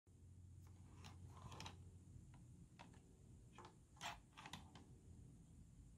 How to press space bar
Instructional video on how to correctly press the space bar.